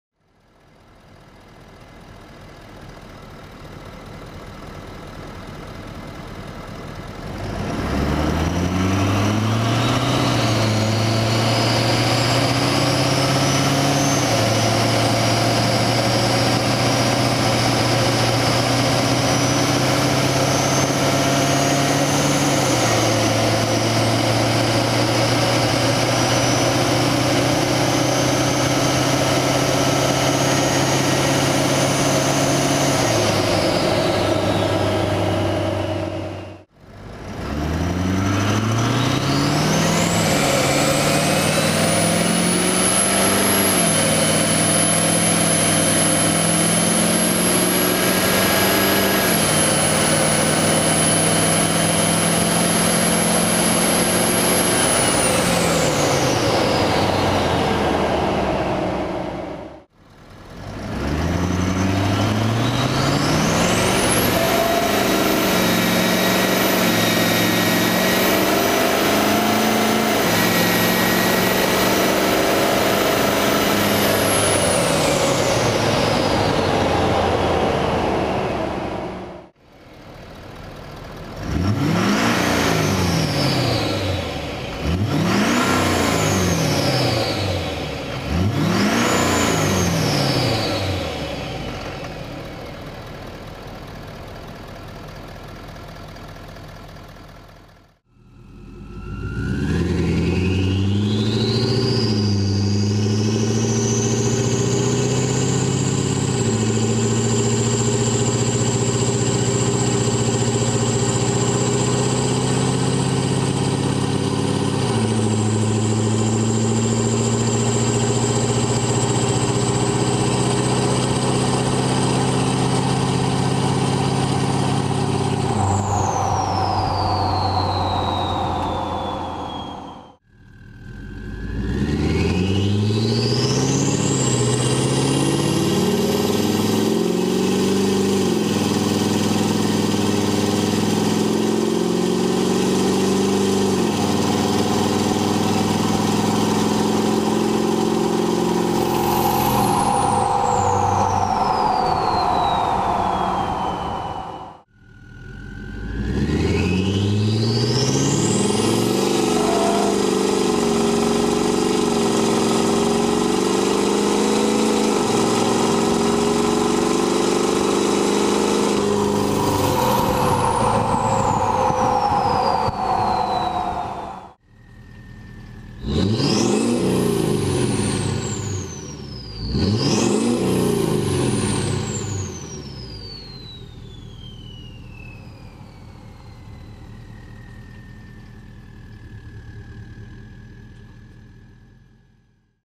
H&S Motorsports 6.7L Power Stroke Turbo Kit Sound
We get a lot of people asking what our Turbo Kit sounds like installed on a 6.7L Power Stroke, so we made this quick video to demonstrate the sound under different driving conditions. The truck is equipped with a 5" exhaust and muffler.